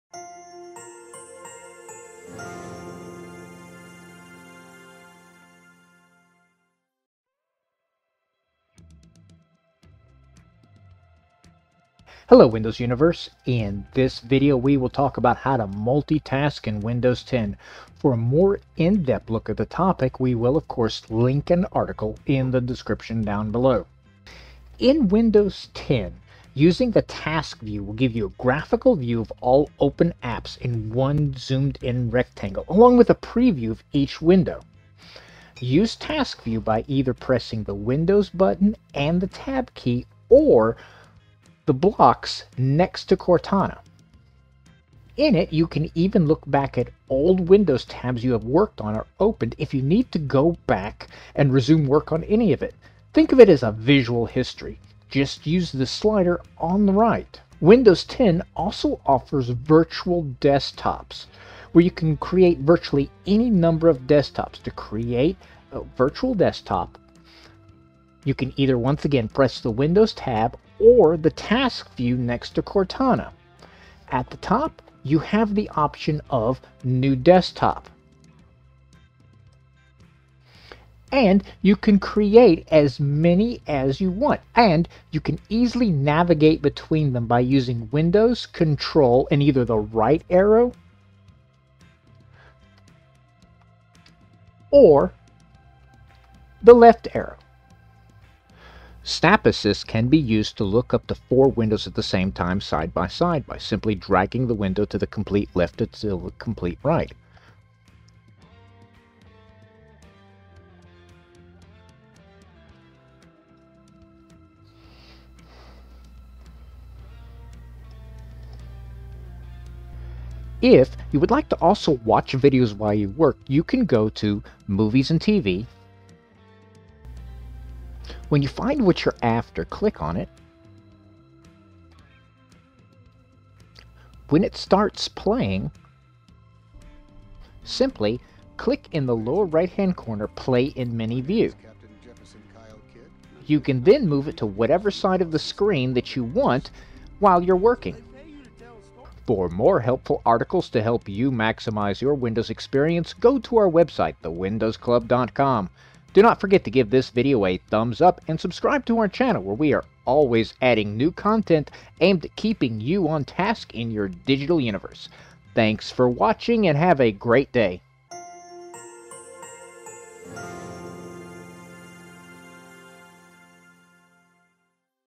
How to Multitask in Windows 10 like a Pro
Multitasking has grown so much into us that we have actually forgotten that we are doing it. Any veteran Windows user would be impressed on how much Windows 10 has improved starting from Windows 95. Not only managing multiple apps are now much easier, but you get to see a live preview of all of your windows.

In this post, I am talking you some tips, on how you can beautifully manage your multiple windows, multitask in a number of ways, and save a lot of time & be more productive in tight situations.

1] Task View instead of ALT + TAB

2] Do not have a Second Monitor? Use Virtual Desktops

3] Stack Windows Side by Side with Snap Assist

4] You can also Scroll Inactive Windows!

5] Love Watching Videos when working? Mini Player is here to help